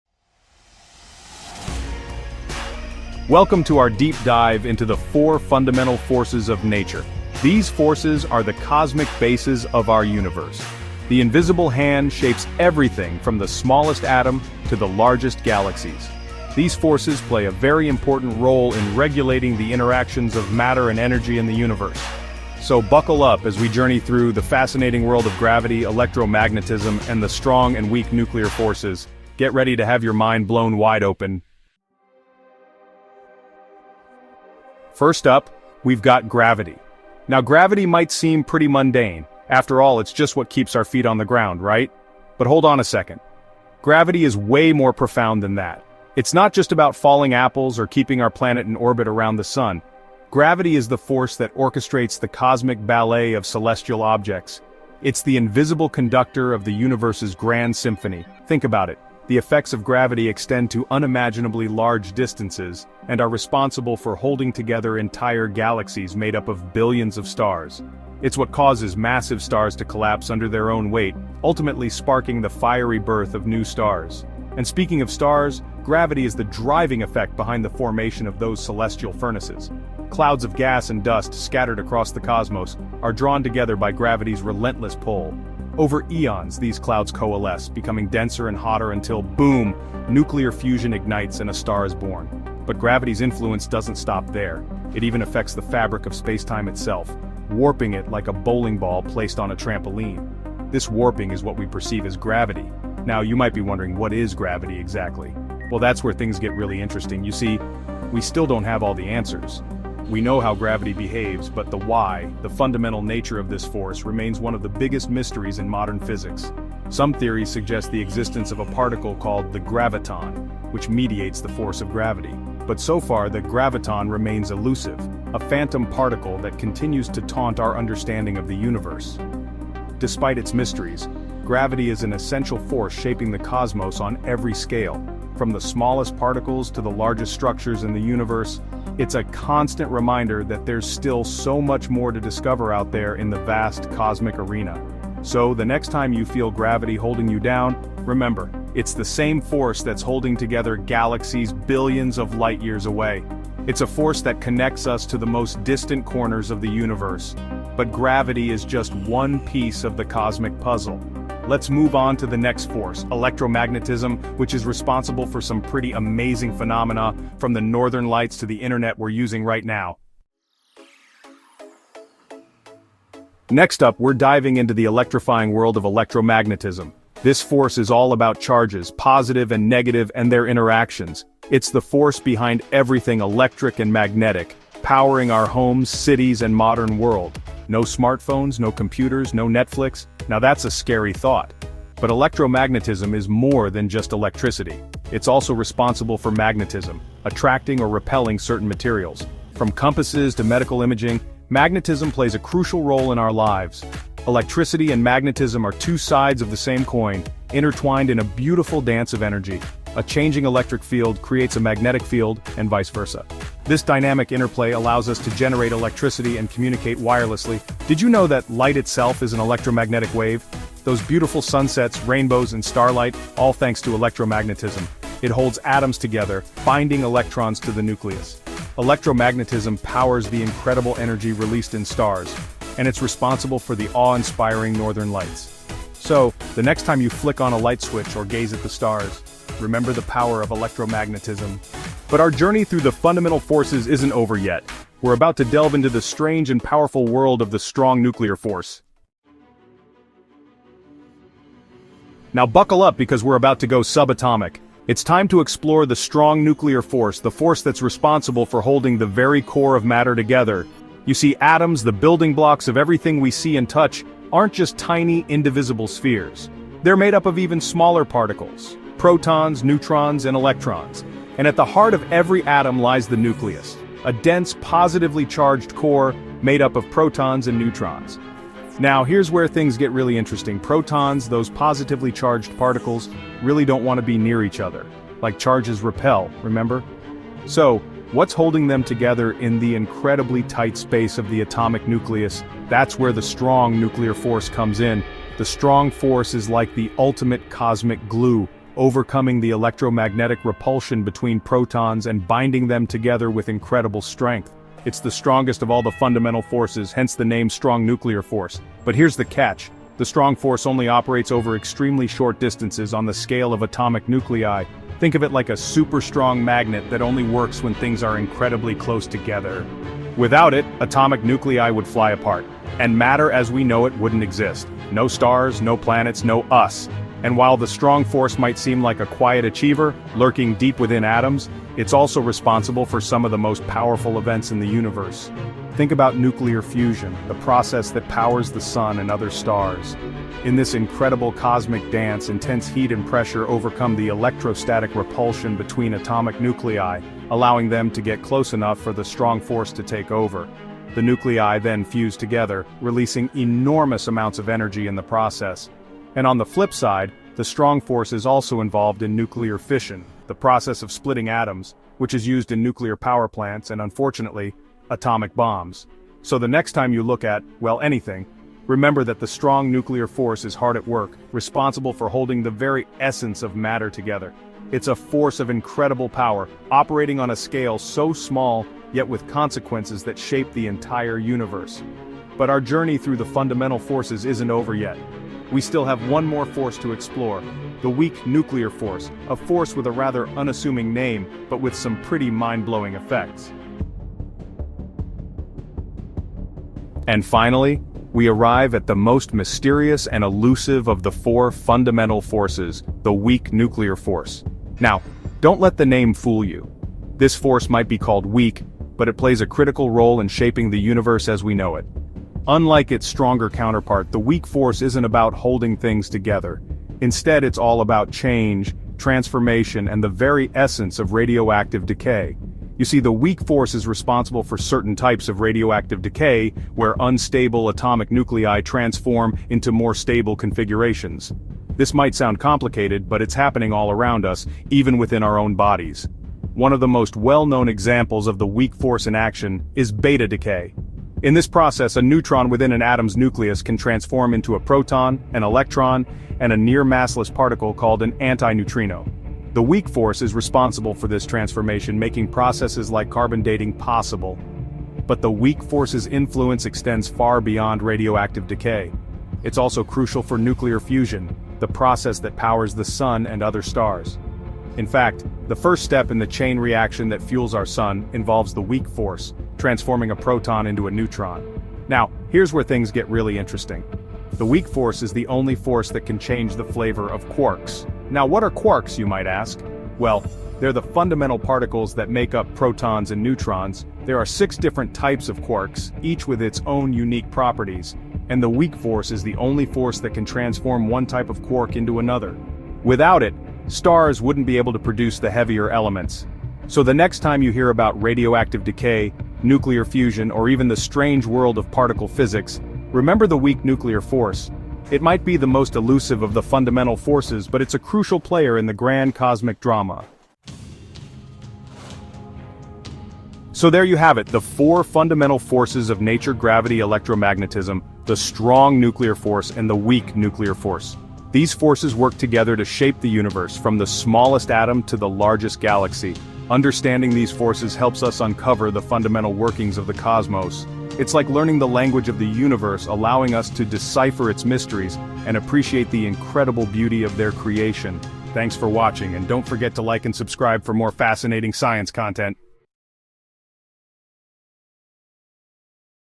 The Four Fundamental Forces of Nature Explained!
Discover the incredible forces that shape our universe in "The Four Fundamental Forces of Nature Explained!" 🌌 From the gravitational pull that keeps our feet on the ground to the electric and magnetic interactions that illuminate our world, this video delves into the unique characteristics and roles of each force. Explore how the strong nuclear force binds atomic nuclei together and how the weak nuclear force drives fundamental processes, including radioactive decay.

Join us as we explore the interplay of these forces that govern everything from the motion of galaxies to the behavior of particles within atoms.

OUTLINE:

00:00:00 The Four Fundamental Forces of Nature Explained!
00:00:31 Unveiling Nature's Unifying Force
00:02:50 Electromagnetism and Its Electrifying Effects
00:04:09 Strong Nuclear Force - The Tiniest Giant
00:06:33 Weak Nuclear Force - The Master of Transformation
00:08:30 A Cosmic Symphony of Power and Elegance